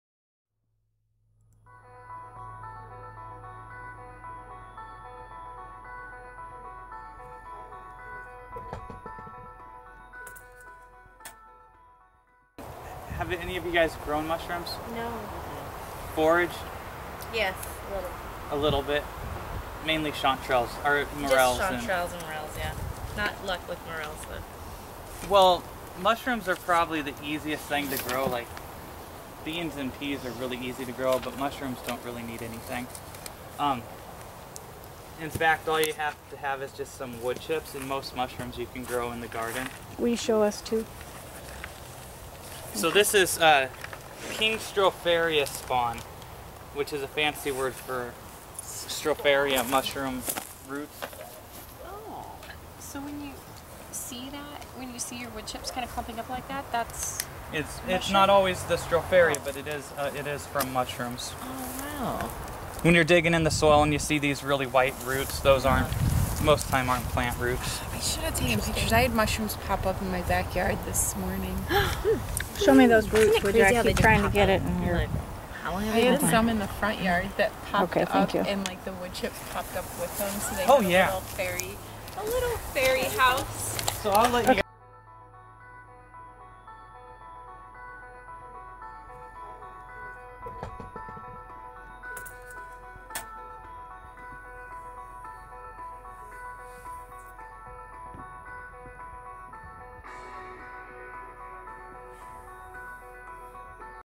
Matt and the Mushrooms Number one
First in a series of 4 videos from Olivia Park School Garden.  More will be uploaded later.  They will be longer.